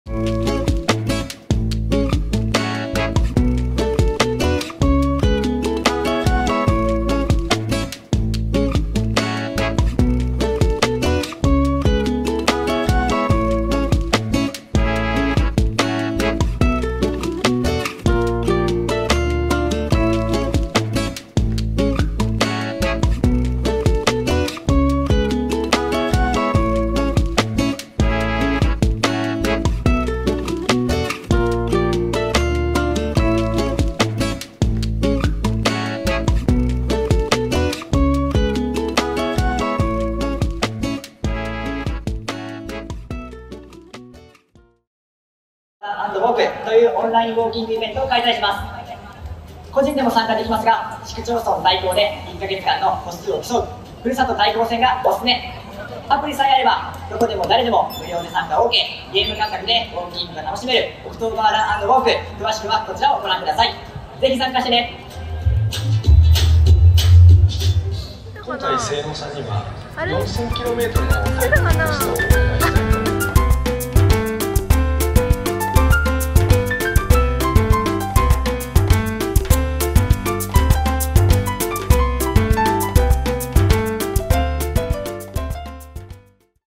【案件映像をチェック＆岐阜を散策♪ / Vlog #19】
サッカーや陸上競技でお馴染みの、岐阜メモリアルセンター長良川競技場へランニングしてきました！
当日はJ3リーグの試合「FC岐阜vsSC相模原」が開催。制作させていただいた映像が試合中のスコアボードに映し出されるとのことで緊張とワクワクでした^^;